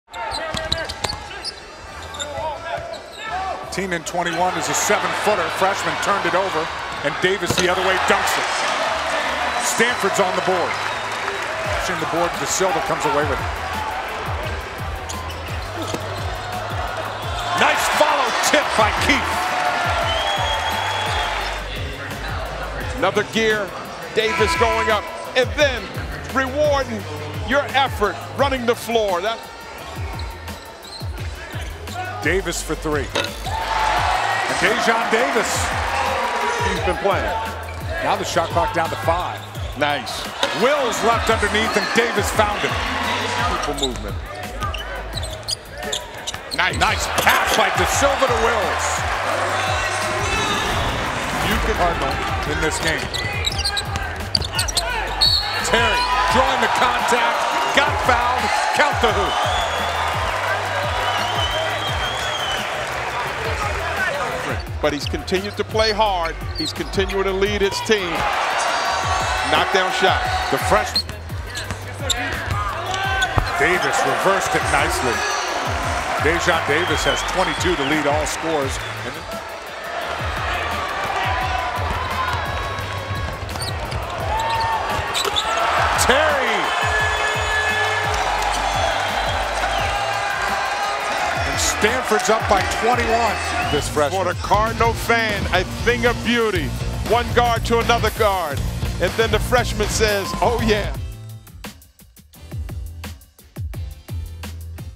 Stanford Men's Basketball vs. Cal [1.2.20]
Stanford men's basketball started Pac-12 action by defeating rival California 68-52 on Thursday night in Stanford. Daejon Davis led the way for the Cardinal with a team-high 20 points and six rebounds. It was the juniors' first 20-point game of the season.